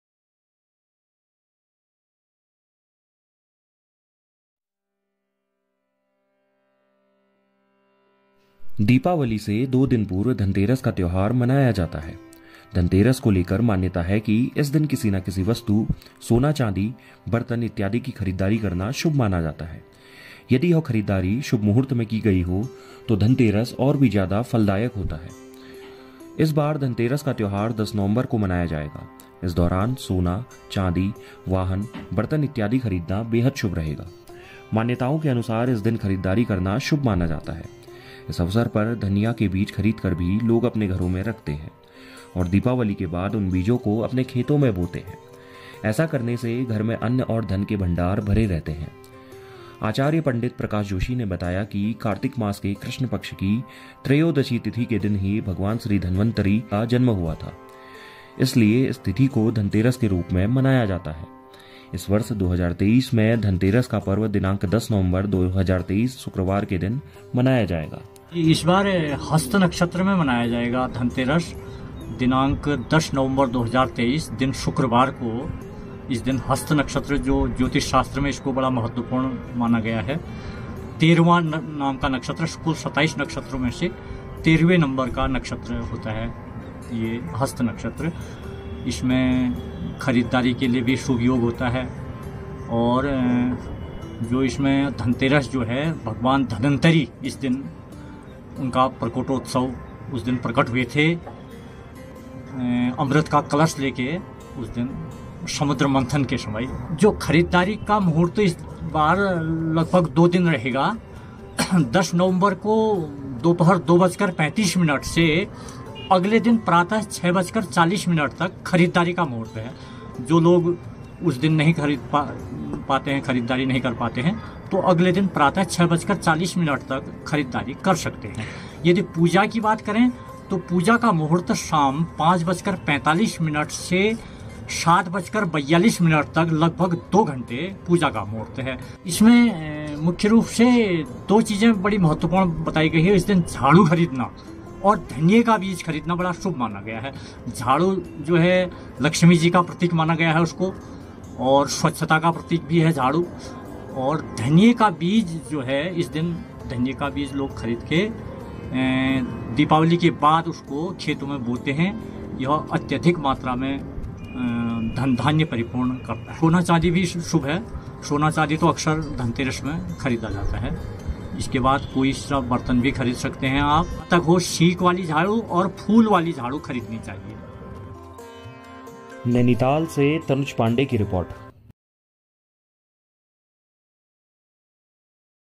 Dhanteras 2023: धनतेरस पर बन रहा शुभ संयोग, जानें खरीदारी और पूजा का शुभ मुहूर्त
Diwali Special: दीपावली से दो दिन पूर्व धनतेरस का त्योहार मनाया जाता है. धनतेरस को लेकर मान्यता है कि इस दिन किसी न किसी वस्तु, सोना चांदी, बर्तन, वाहन इत्यादि की खरीदारी शुभ मानी जाती है. यदि इस दिन कोई अद्भुत संयोग हो तो ये दिन और भी शुभ माना जाता है. इस बार धनतेरस का त्योहार 10 नवंबर को मनाया जाएगा.
Diwali Special: The festival of Dhanteras is celebrated two days before Diwali. Regarding Dhanteras, there is a belief that purchasing any item, gold, silver, utensils, vehicles etc. on this day is considered auspicious. If some wonderful coincidence happens on this day, then this day is considered even more auspicious. This time the festival of Dhanteras will be celebrated on 10th November.